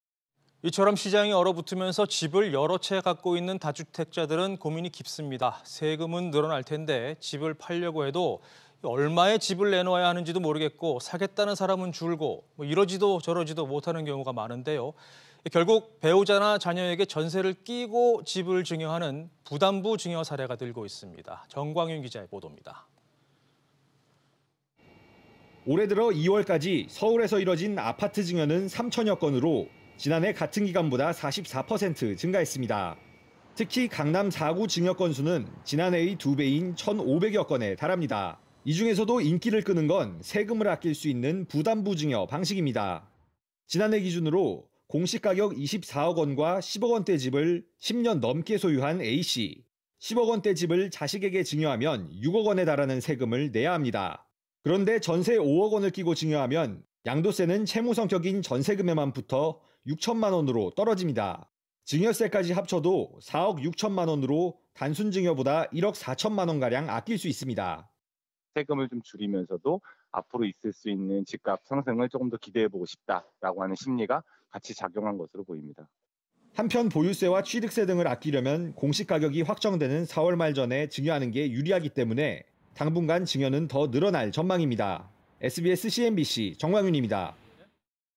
세부담 줄이려고 전세끼고…다주택자의 ‘부담부 증여’
[앵커]

시장이 얼어붙으면서 집을 여러 채 가진 다주택자들은 고민이 깊습니다.

세금은 늘어날 텐데 집을 팔려고 해도 얼마에 내놓아야 하는지도 모르겠고, 사겠다는 사람은 줄어드니 이러지도 저러지도 못하는 경우가 많은데요.

결국 배우자나 자녀에게 전세를 끼고 집을 증여하는 '부담부 증여' 사례가 늘고 있습니다.

정광윤 기자가 보도합니다.